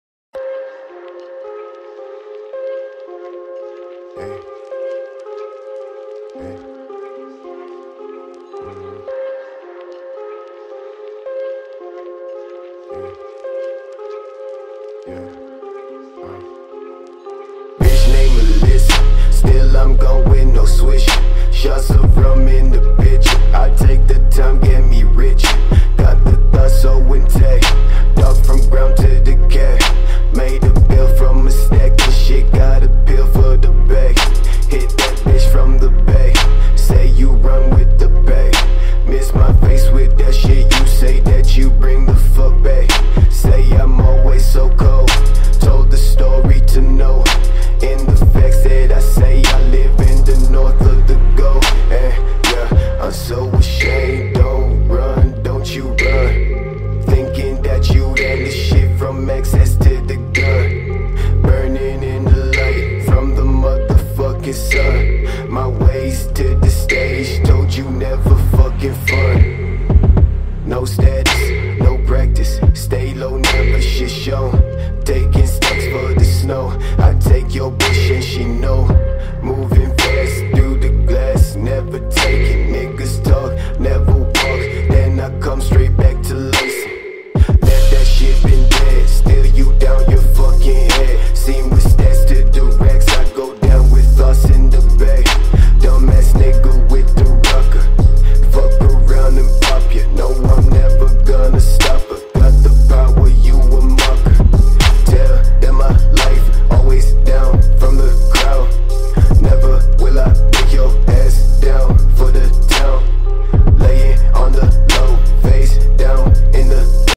Audio Visual logo Animation - Night Lovell
Logo Animation audio visual created in after effects | Alex Fasui